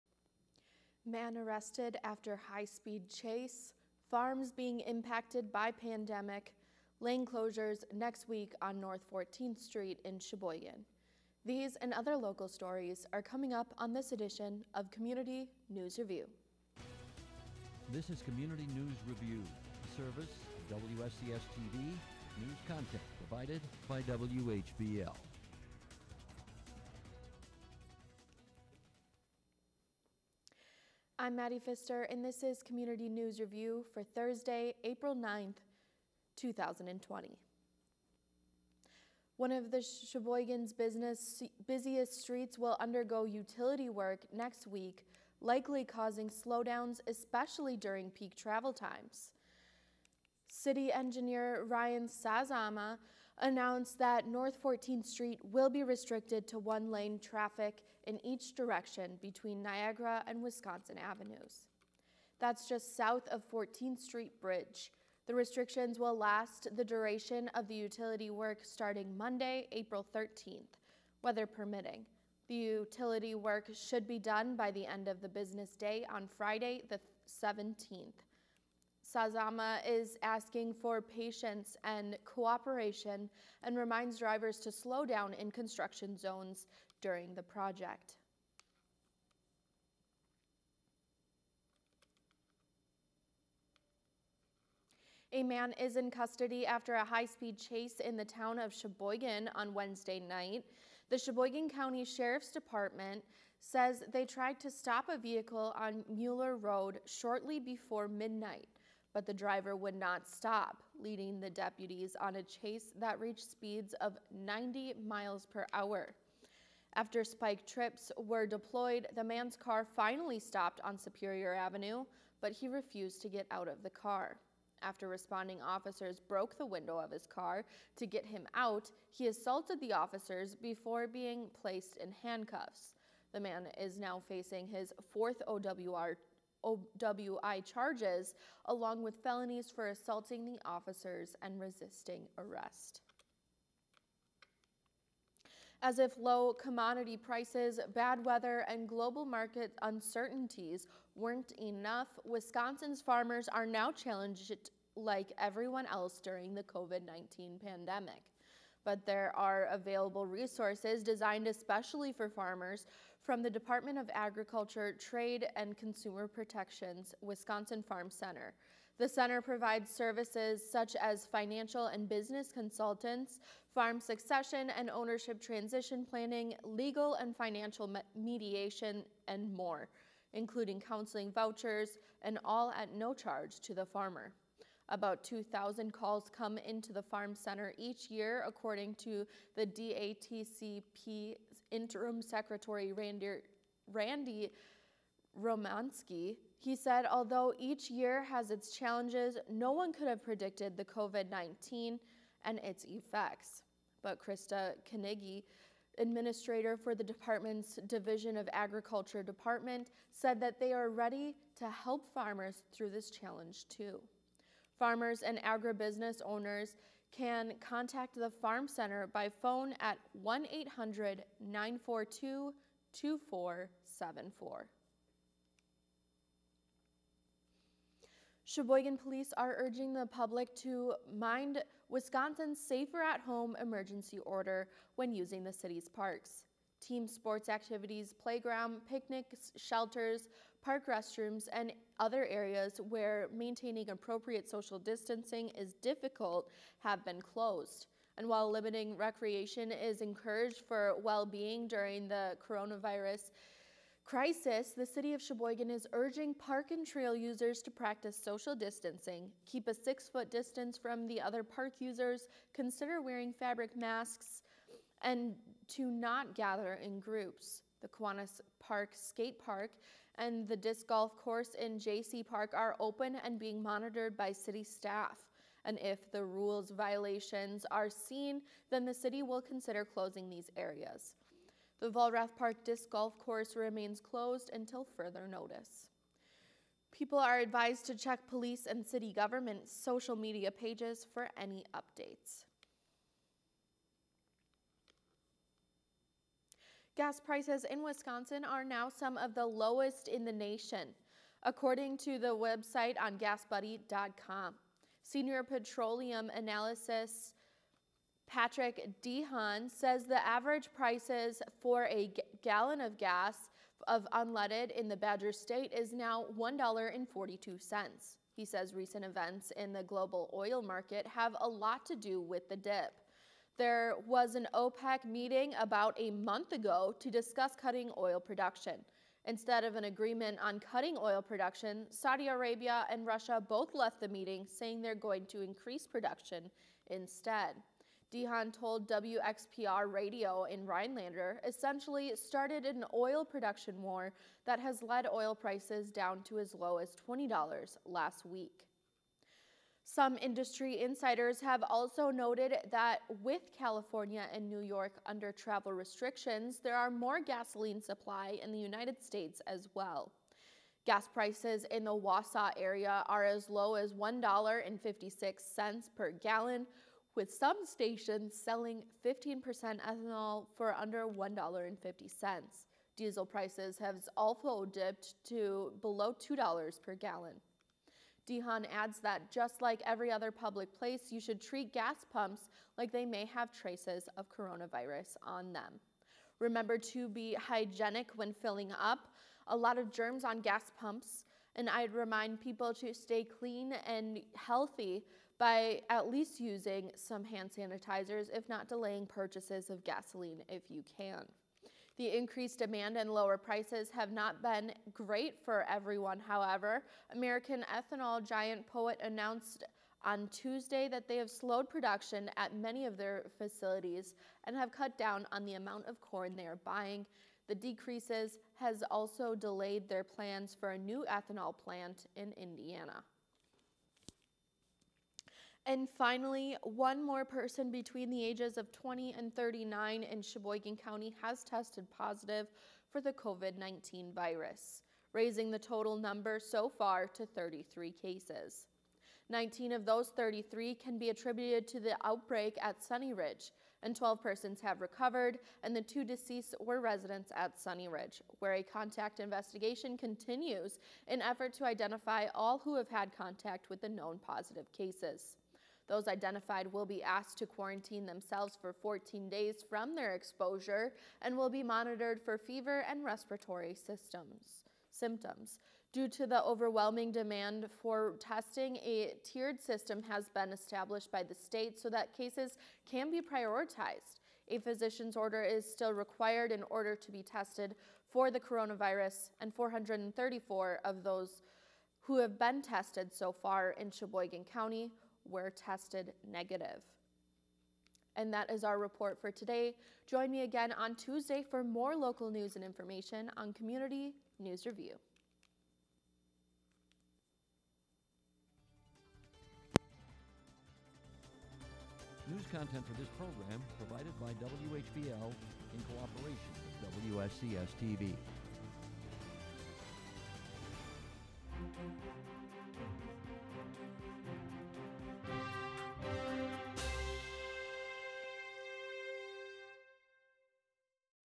CNR 0409202
Sheboygan news digest, April 9, 2020.